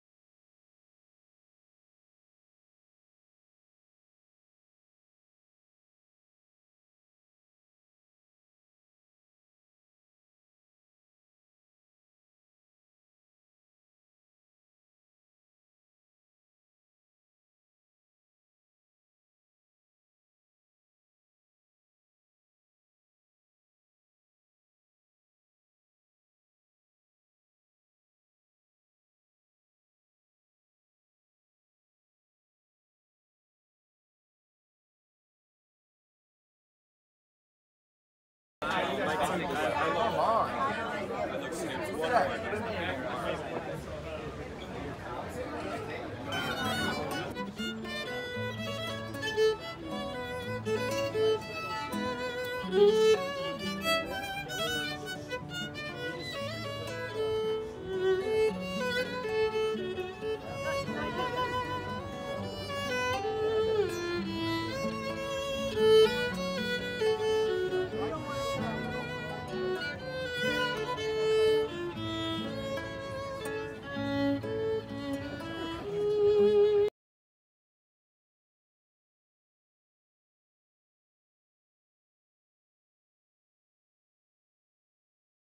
Beautiful Wedding at Block Island Jessica Haddy - KRUTZ Strings Ambassador
What a gorgeous wedding at Block Island, Rhode Island!

Closer to the end, enjoy Jessica Haddy (KRUTZ Strings Ambassador) play alongside her husband.